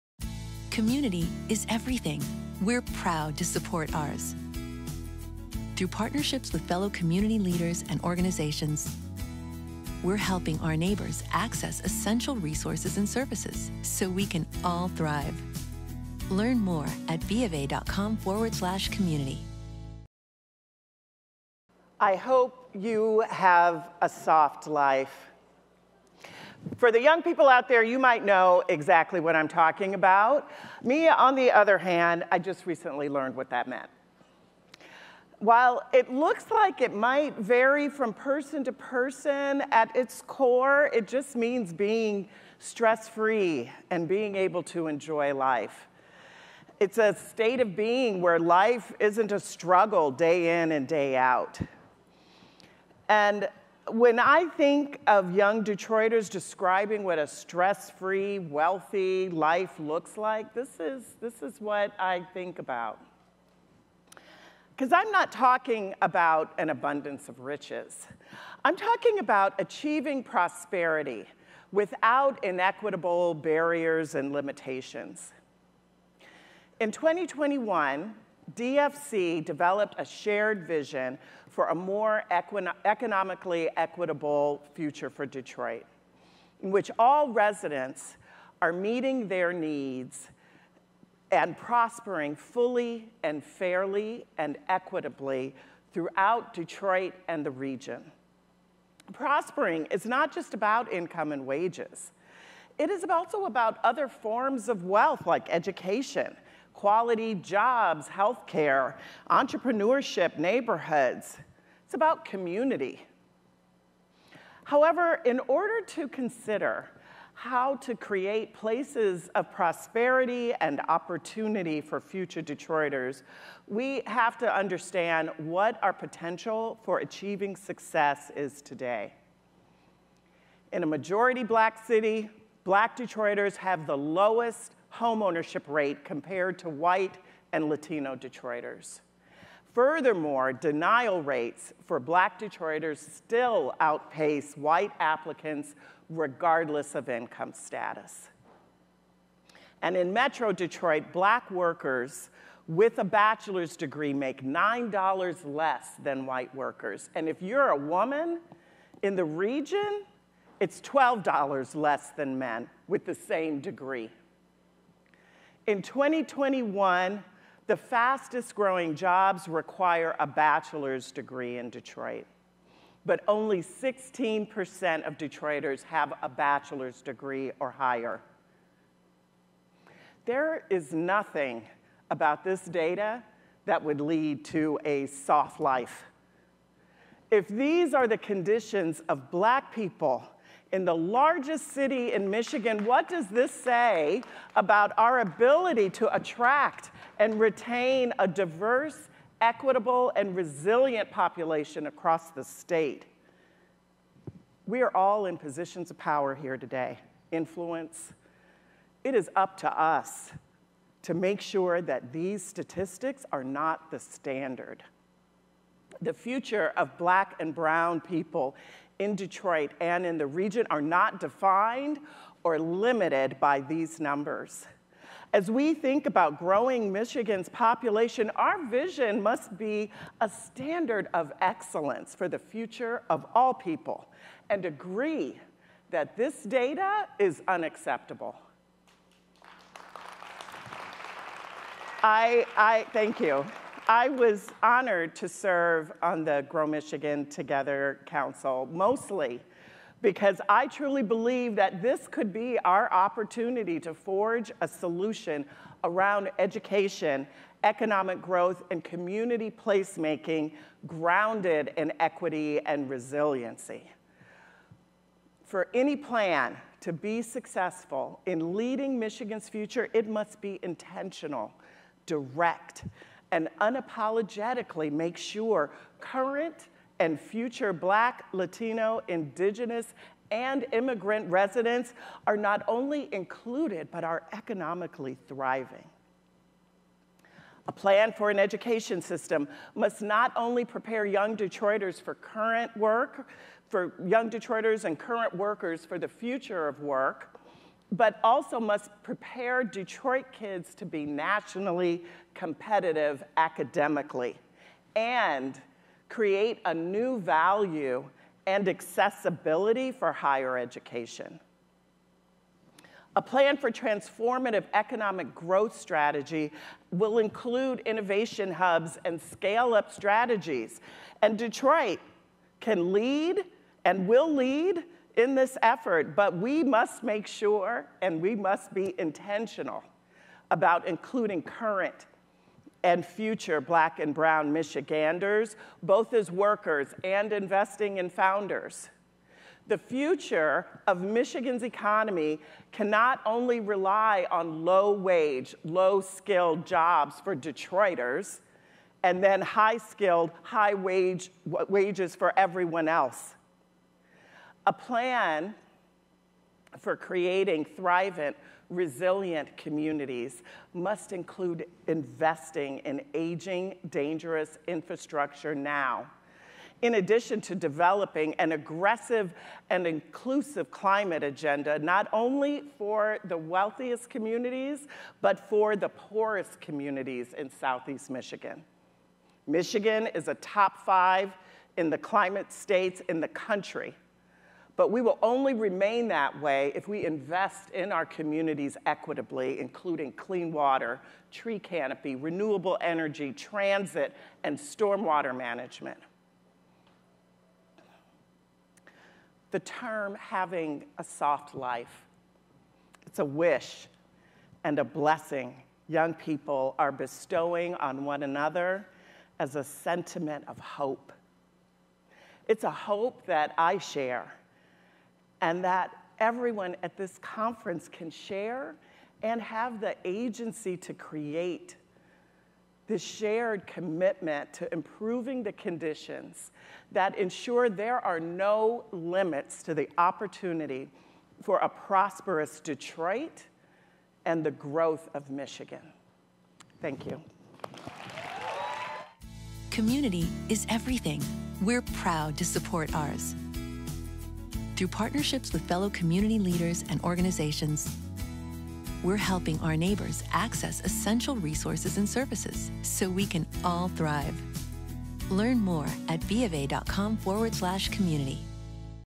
2024 Detroit Policy Conference: Power Perspective - Anika Goss
Speaker: Anika Goss, Chief Executive Officer, Detroit Future City